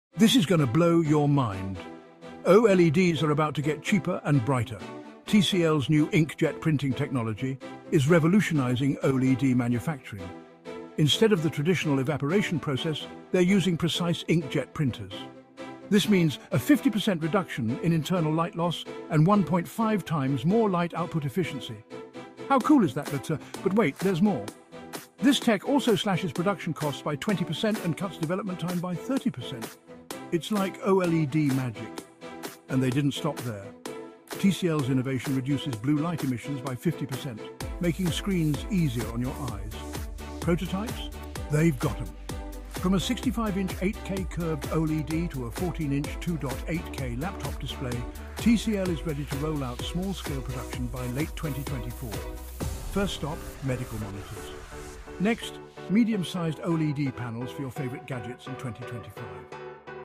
TCL Revolutionizes OLED Tech with Inkjet Printing!
TCL's display manufacturing arm CSOT revolutionizes OLED production with innovative inkjet printing technology, achieving 50 percent less light loss and 1.5x better efficiency. Set for production in late 2024, this breakthrough promises cheaper, better displays while cutting manufacturing costs by 20 percent.

CSOT, which is TCL's manufacturing arm, has been making some solid strides with its inkjet-printed OLED tech, showing off big gains in efficiency and cutting production costs. Ming-Jong Jou, who heads up the Technology Planning Center, shared these updates at the Omdia Korea Display Conference.

Instead of sticking with the usual evaporation process for making OLEDs, this new method uses precision inkjet printers, switching things up quite a bit. Thanks to this shift, they've managed to cut internal light loss by 50 percent and boost light output efficiency by about 1.5 times.

While inkjet printing is already used in OLED production to add encapsulation layers and apply quantum dots in Samsung's QD-OLEDs, TCL CSOT took it further. They're using it for emitters and other stack materials, which helps them better use their resources—hitting up to 90 percent material efficiency. That's twice as good as the previous ways of doing things. And on top of that, they've reduced blue light emissions by 50 percent.

This technology isn't just about efficiency, though. It's also driving down manufacturing costs by 20 percent and speeding up product development by 30 percent. They've already shown off several implementations of this technology, like a 65-inch 8K curved OLED panel with a 120Hz refresh rate and 1800R curvature, plus a 31-inch 4K dome-shaped OLED and a 14-inch 2.8K laptop display.

The company plans to start small-scale production towards the end of 2024, starting with 21.6-inch 4K OLED panels for medical monitors. Then, in 2025, they're looking to branch out into making medium-sized OLED panels for computing devices, too.